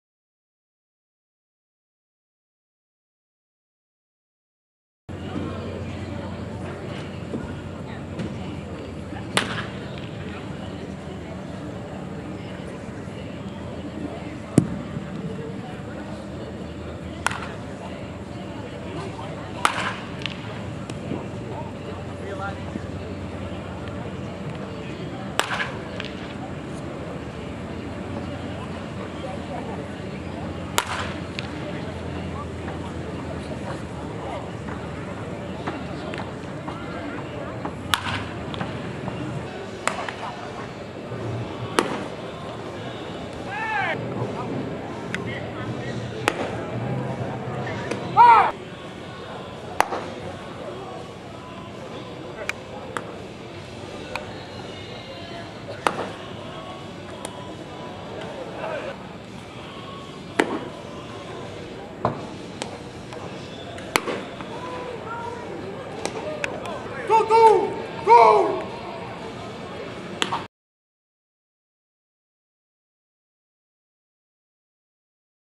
Andrew Dennis - C - Jamestown Jammers (2013-08-21 at Mahoning Valley)
Pittsburgh Pirates C prospect, Andrew Dennis, at Mahoning Valley on August 21st, 2013.